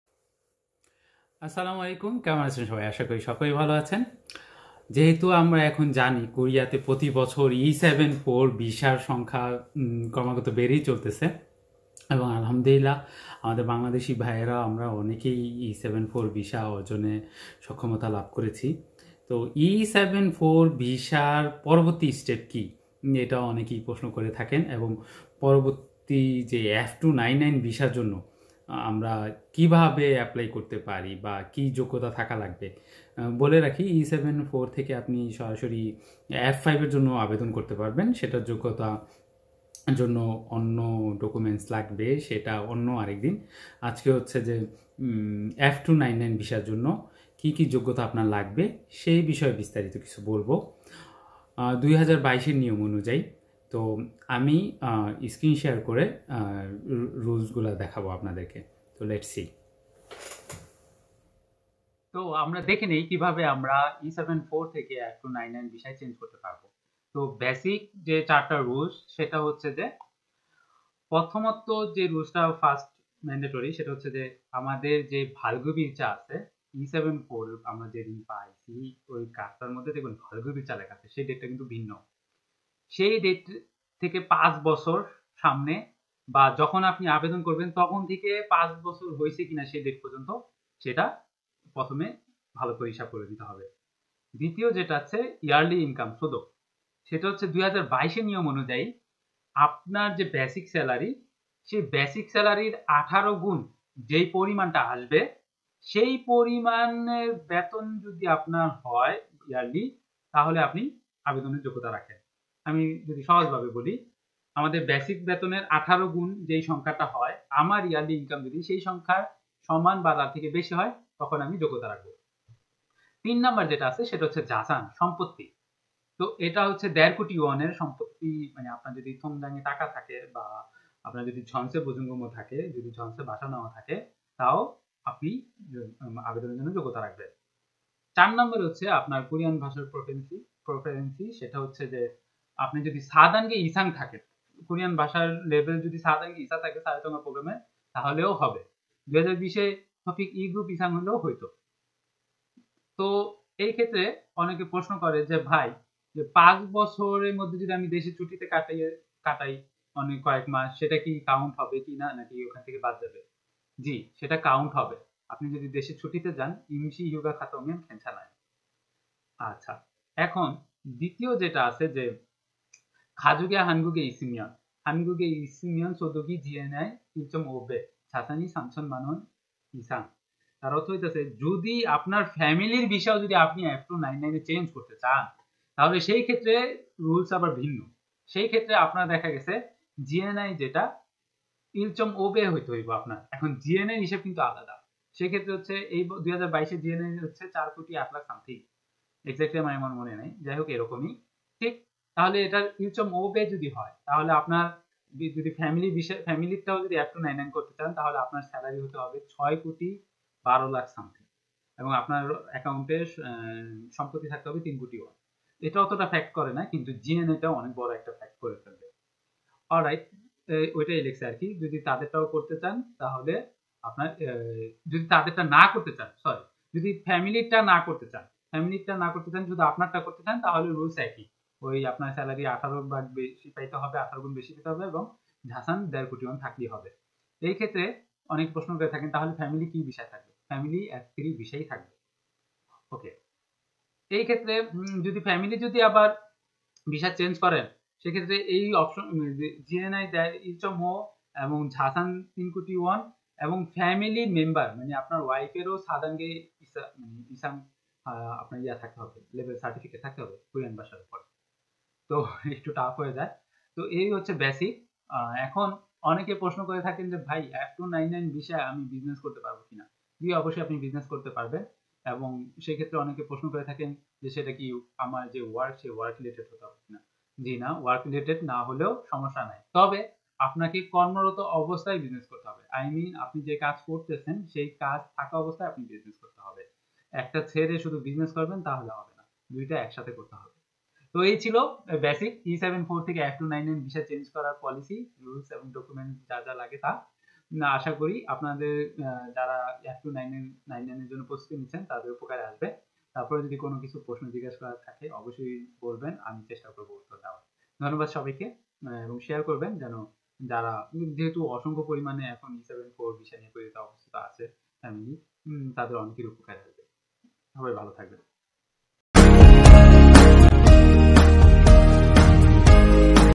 E 7 4 to F 2 99 체류자격 변경
আসসালামুয়ালাইকুম আজকের ভিডিওতে আমরা দেখবো E-7-4  ভিসা থেকে F-2-99 ভিসা করার  নিয়মাবলী এবং যোগ্যতা।

Assalamualaikum, In today's video we will see the terms and condition for changing E-7-4 visa to F-2-99 visa .